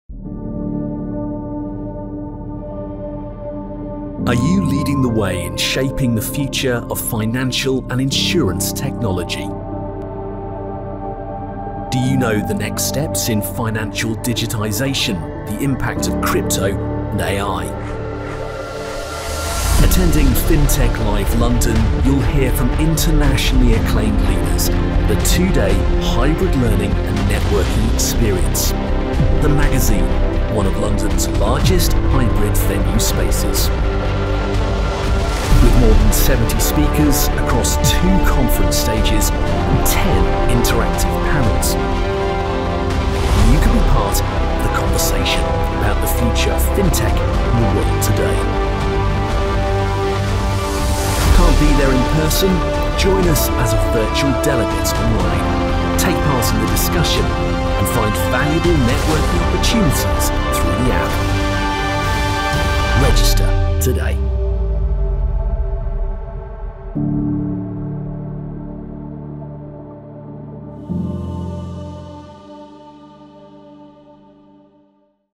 FINTECH LIVE LONDON 2022
Join your peers alongside some of the world’s most innovative financial leaders at FINTECH LIVE LONDON.

FINTECH LIVE LONDON is your opportunity to:
- Connect with the world's largest community of banking and fintech executives to present valuable solutions for, and debate topical issues on, the most pressing aspects of fintech and insurtech in our world today.
- Gain high-level insights on the future of financial technology from over 100 internationally-acclaimed speakers
- Network with your peers, alongside a raft of inspiring leaders, movers and shakers from across the financial services industry